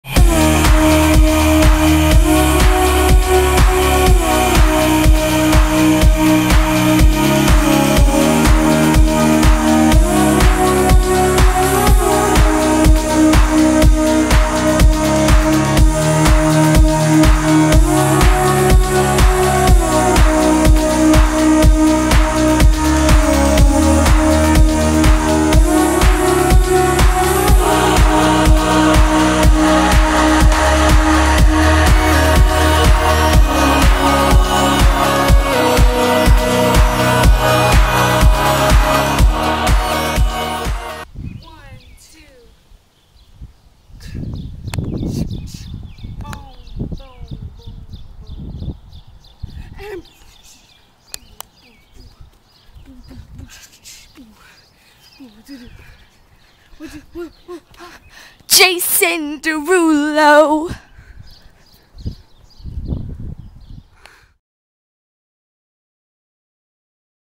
The best dance ever performed in the middle of the road
ever wonder what 2 white girls look like after planning a dance in the middle of the road for 5 mins. well now you know